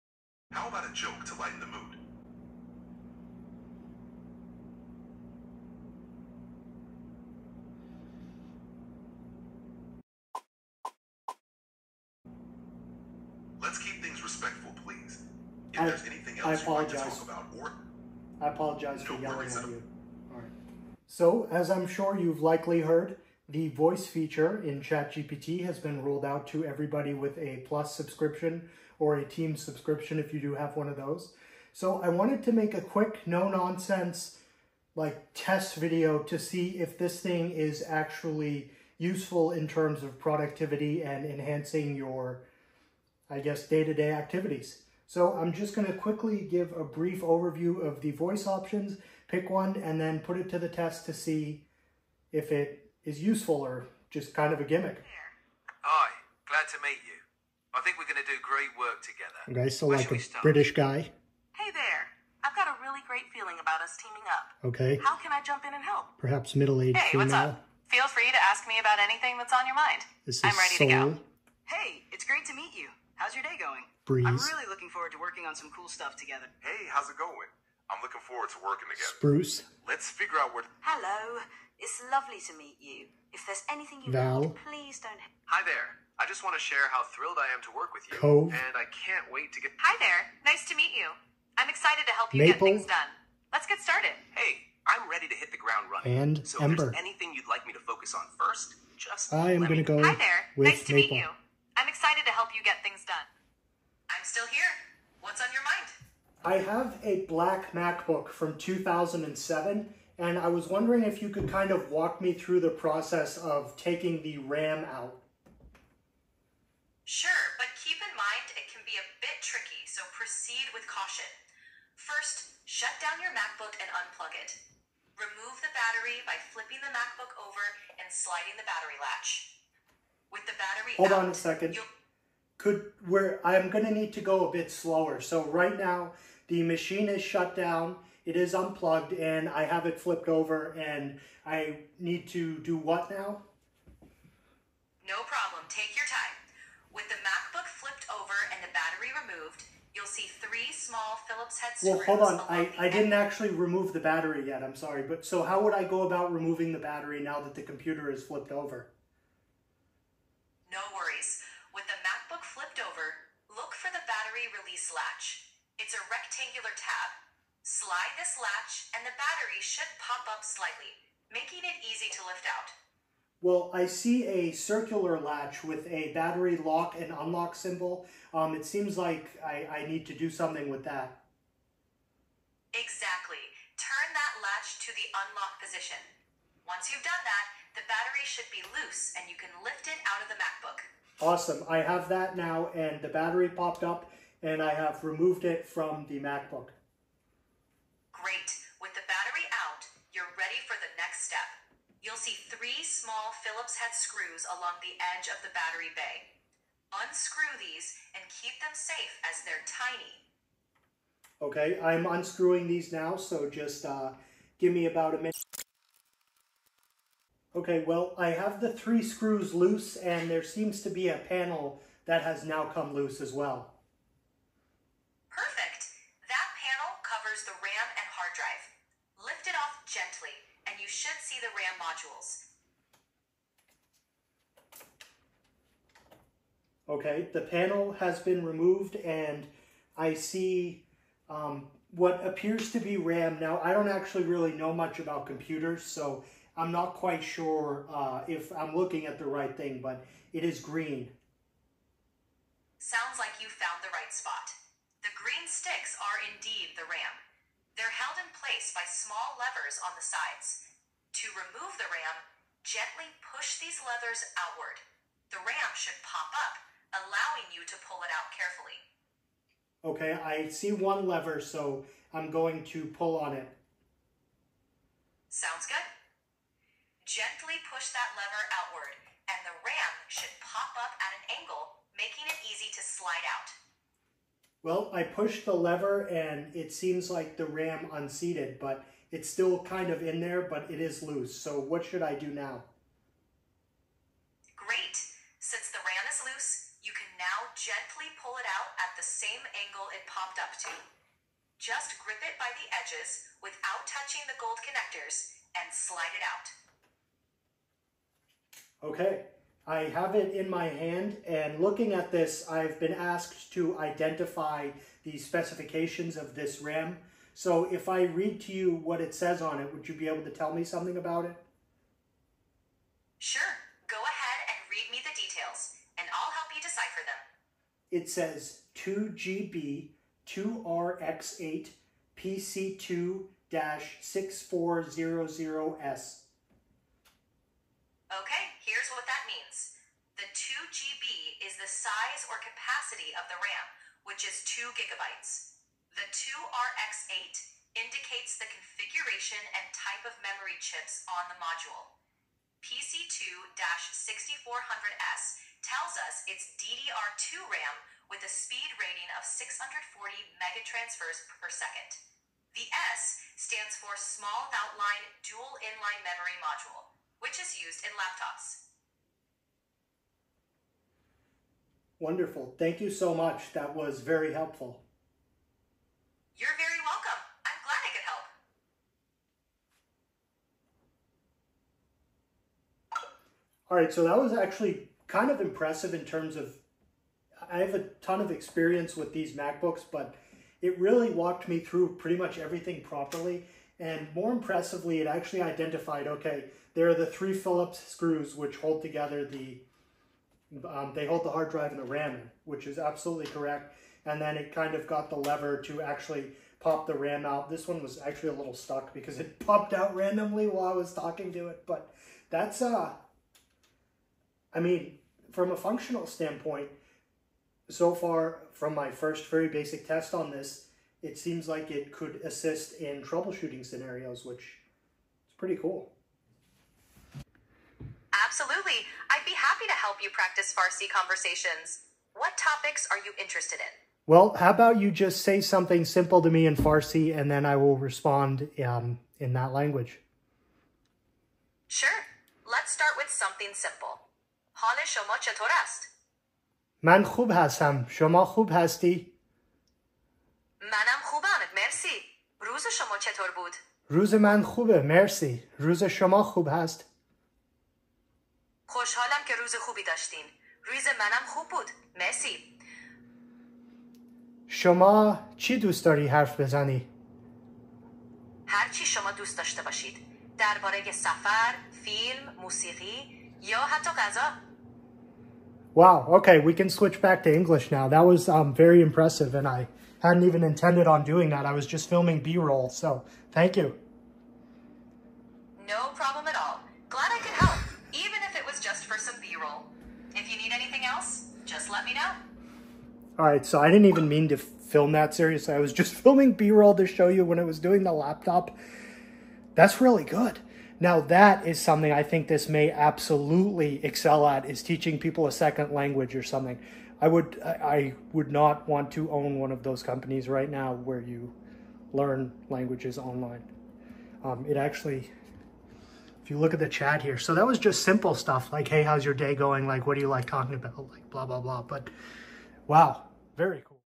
ChatGPT Advanced Voice Test: Gimmick or Game-Changing Tool?
00:00 - Nonsense
00:20 - Intro
00:56 - Voice Options
01:44 - Laptop RAM Removal Walk-Through
07:07 - Speaking A Different Language

In this video, I finally dive into ChatGPT’s advanced voice features, which I had been overlooking for a while. Despite using ChatGPT exclusively through its web interface since December 2022, I decided to try out the voice functionality by downloading the app on my old Google Pixel 5. I explore the available voice options and choose "Maple" to guide me through a hands-on task: removing the RAM from a 2007 MacBook.

During the process, ChatGPT effectively walks me through the steps in a conversational style, and I test its ability to assist someone unfamiliar with this type of hardware task. As a bonus, I also engage in a short conversation in Farsi, demonstrating ChatGPT’s impressive multilingual capabilities. The video documents how well the voice feature works, both for practical instruction and language learning, offering a glimpse of the potential educational value this technology holds.